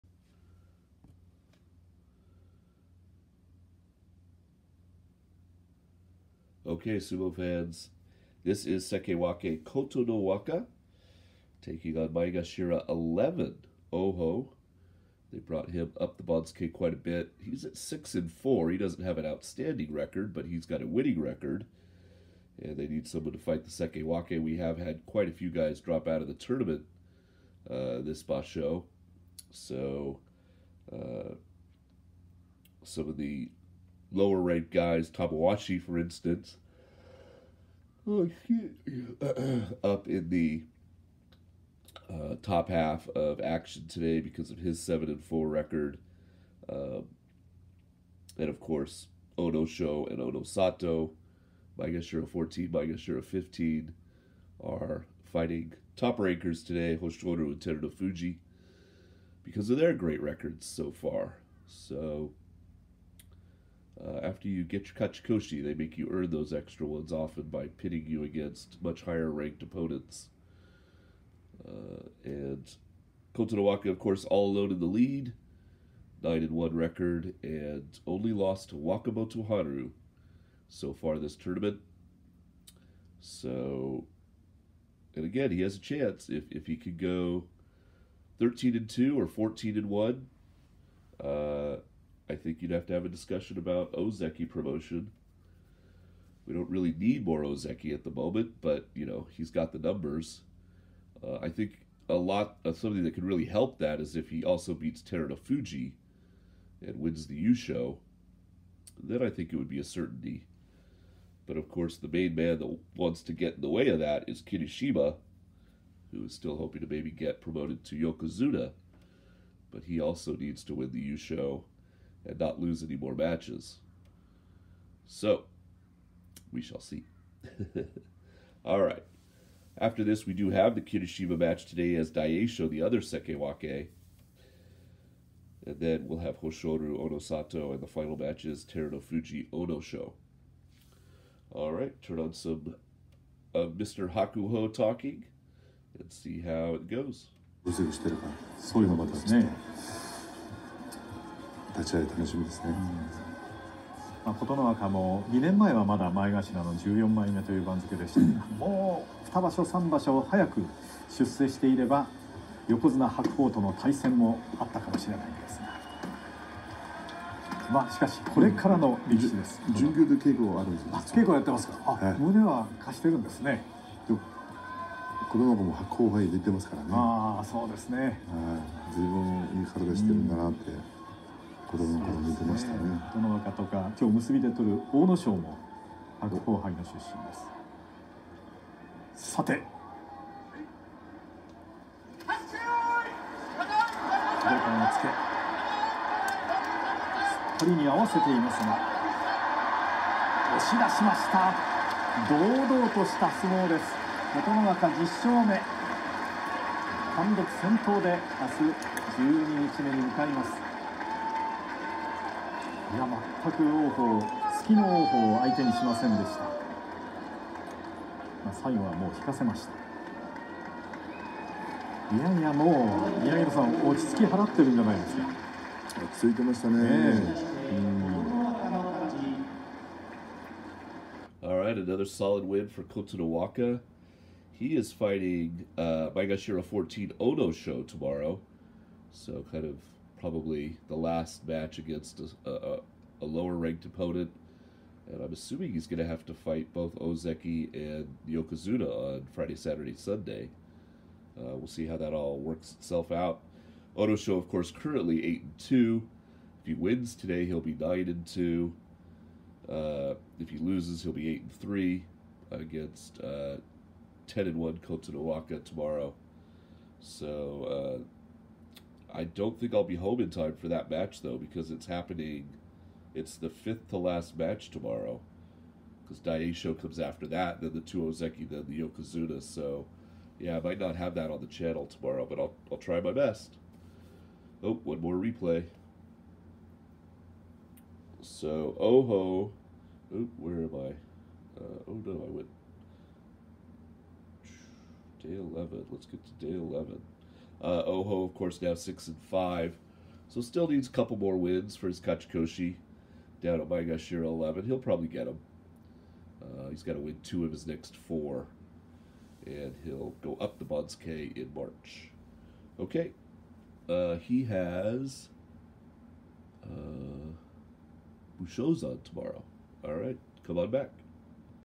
January 2024 - Day 11 - Kotonowaka v Oho
Sekiwake Kotonowaka (9-1) is all alone at the top of the leaderboard after 10 days.  He goes for double-digit wins here on Day 11 against M11 Oho (6-4).

-Jason